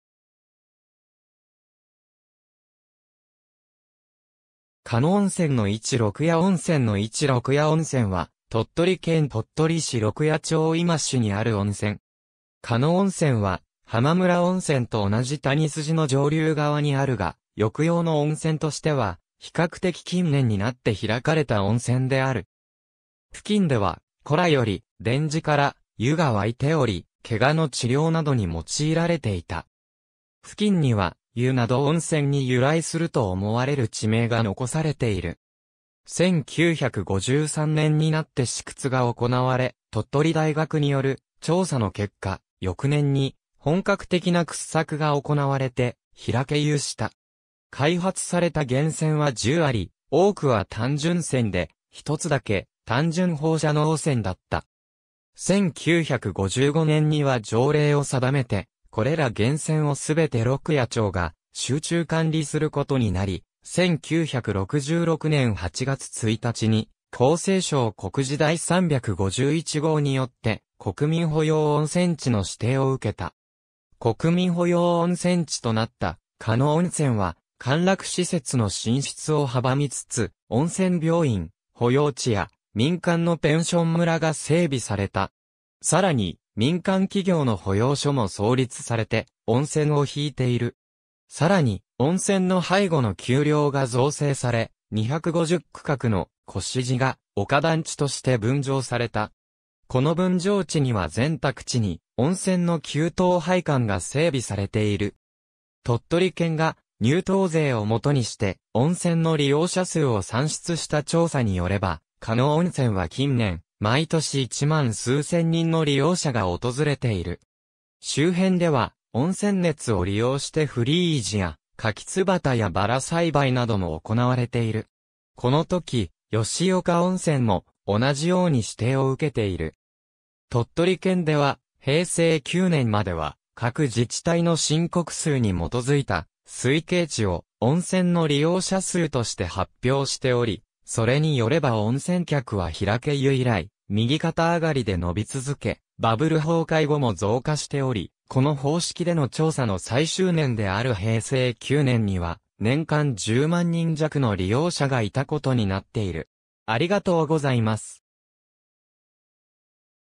鹿野温泉
鹿野温泉の位置 鹿野温泉の位置 鹿野温泉（しかのおんせん）は、鳥取県鳥取市鹿野町今市（旧鹿野町）にある温泉。
鹿野温泉は浜村温泉と同じ谷筋の上流側にあるが、浴用の温泉としては比較的近年になって開かれた温泉である。
付近では古来より田地から湯が湧いており、怪我の治療などに用いられていた。
付近には「湯」など温泉に由来すると思われる地名が残されている。
1953年（昭和28年）になって試掘が行われ、鳥取大学による調査の結果、翌年に本格的な掘削が行われて開湯した。
開発された源泉は10あり、多くは単純泉で、1つだけ単純放射能泉だった。
1955年（昭和31年）には条例を定めてこれら源泉をすべて鹿野町が集中管理することになり、1966年（昭和41年）8月1日に厚生省告示第351号によって国民保養温泉地の指定を受けた。
国民保養温泉地となった鹿野温泉は、歓楽施設の進出を阻みつつ、温泉病院、保養地や民間のペンション村が整備された。
さらに民間企業の保養所も創立されて温泉を引いている。
さらに温泉の背後の丘陵が造成され、250区画の越路ヶ丘団地として分譲された。
この分譲地には全宅地に温泉の給湯配管が整備されている。
鳥取県が入湯税を基にして温泉の利用者数を算出した調査に拠れば、鹿野温泉は近年、毎年1万数千人の利用者が訪れている。
周辺では温泉熱を利用してフリージア、カキツバタやバラ栽培なども行われている。
^ このとき吉岡温泉も同じように指定を受けている。
^ 鳥取県では、平成9年までは各自治体の申告数に基づいた推計値を温泉の利用者数として発表しており、それに拠れば温泉客は開湯以来右肩上がりで伸び続け、バブル崩壊後も増加しており、この方式での調査の最終年である平成9年には年間10万人弱の利用者がいたことになっている。